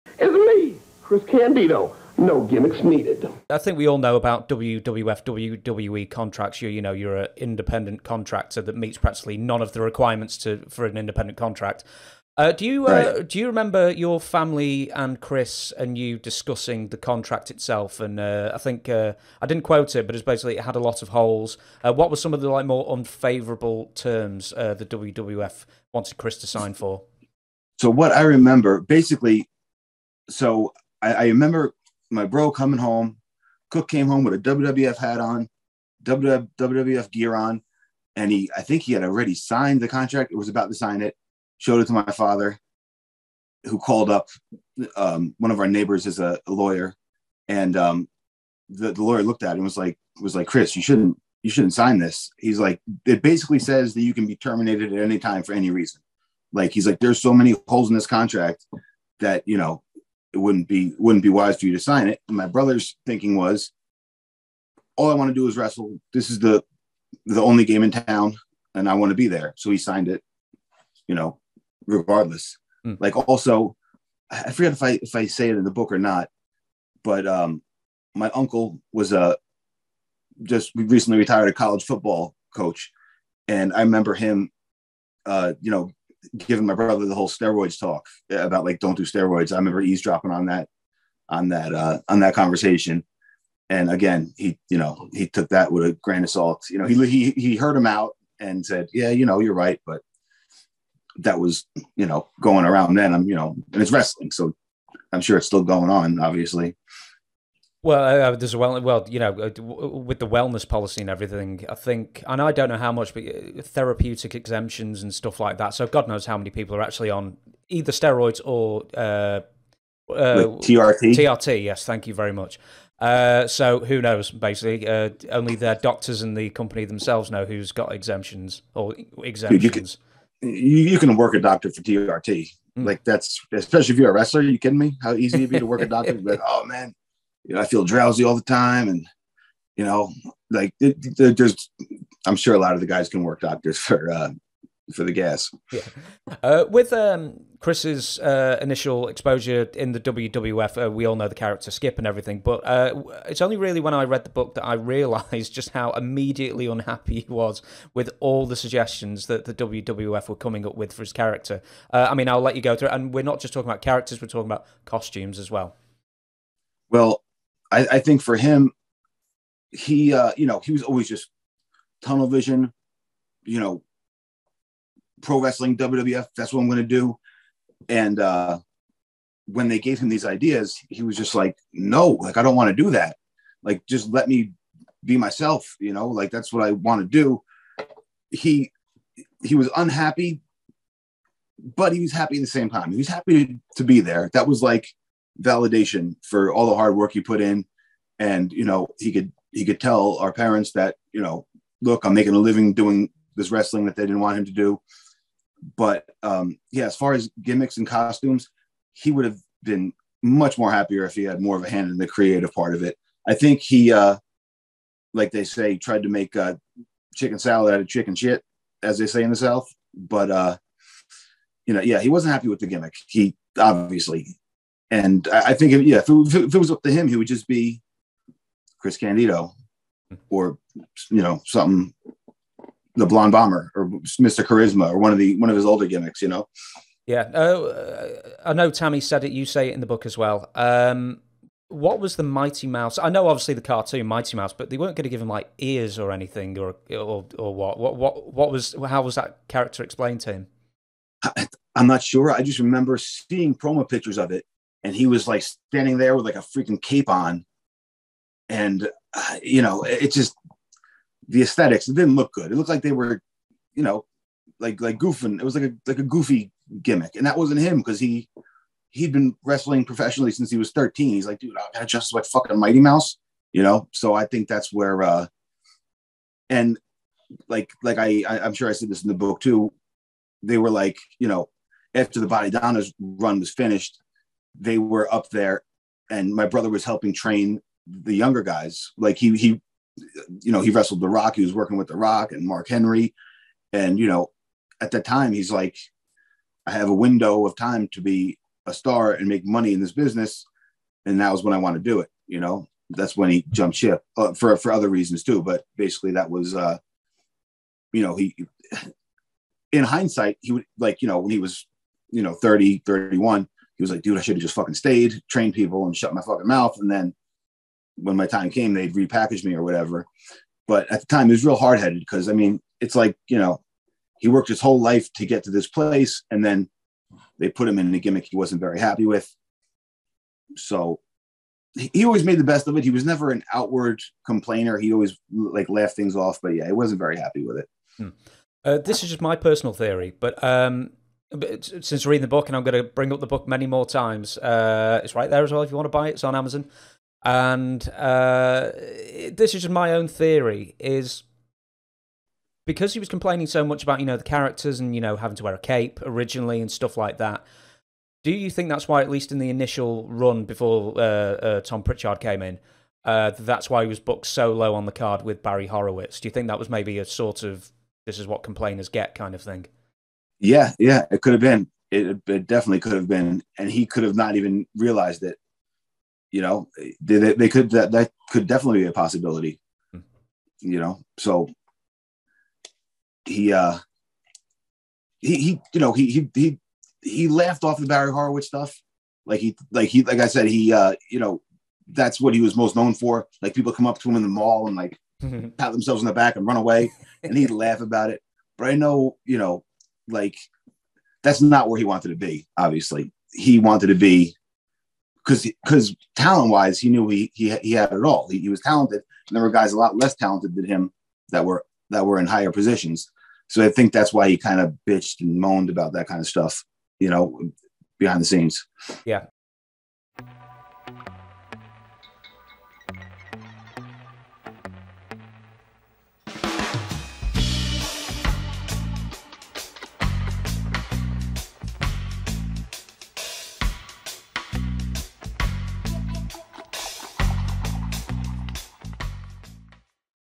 Jonny Candido on Chris HATING Early WWF Run | Mighty Mouse Gimmick & Jobbed Out to Barry Horowitz
Chris Candido: No Gimmicks Needed by Jonny Candido & John Cosper